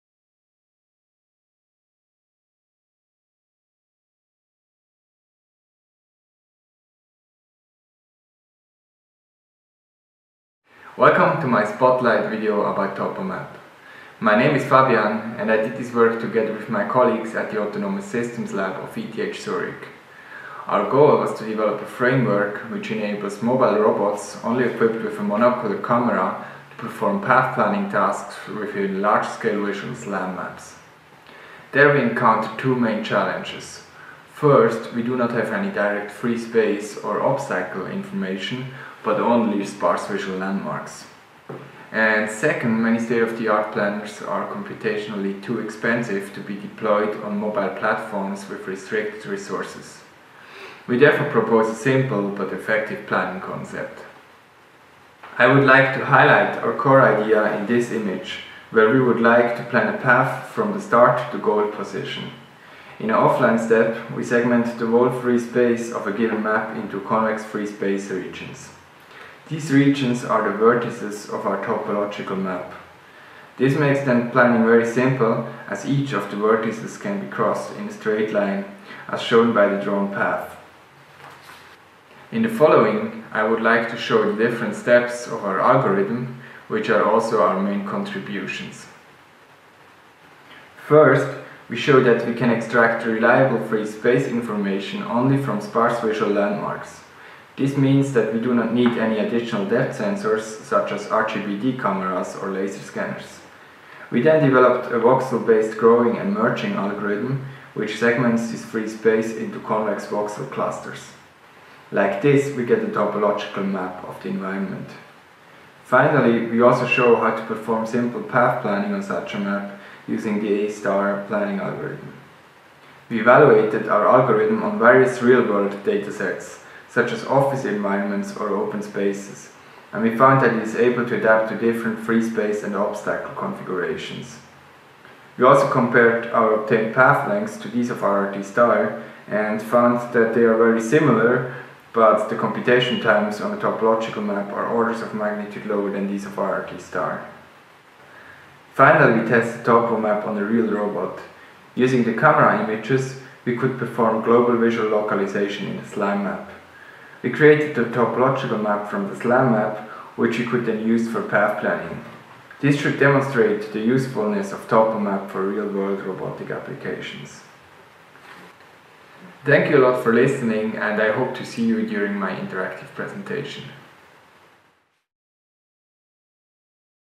Topomap: Topological Mapping and Navigation Based on Visual SLAM Maps
Interactive Session Wed AM Pod U.2
Authors: Blöchliger, Fabian; Fehr, Marius; Dymczyk, Marcin Tomasz; Schneider, Thomas; Siegwart, Roland
Title: Topomap: Topological Mapping and Navigation Based on Visual SLAM Maps

Abstract:
Visual robot navigation within large-scale, semi-structured environments deals with various challenges such as computation intensive path planning algorithms or insufficient knowledge about traversable spaces. Moreover, many state-of-the-art navigation approaches only operate locally instead of gaining a more conceptual understanding of the planning objective. This limits the complexity of tasks a robot can accomplish and makes it harder to deal with uncertainties that are present in the context of real-time robotics applications. In this work, we present Topomap, a framework which simplifies the navigation task by providing a map to the robot which is tailored for path planning use. This novel approach transforms a sparse feature-based map from a visual Simultaneous Localization And Mapping (SLAM) system into a three-dimensional topological map. This is done in two steps. First, we extract occupancy information directly from the noisy sparse point cloud. Then, we create a set of convex free-space clusters, which are the vertices of the topological map. We show that this representation improves the efficiency of global planning, and we provide a complete derivation of our algorithm. Planning experiments on real world data demonstrate that we achieve similar performance as RRT* with significantly lower computation times and storage requirements. Finally, we test our algorithm on a mobile robotic platform to prove its advantages.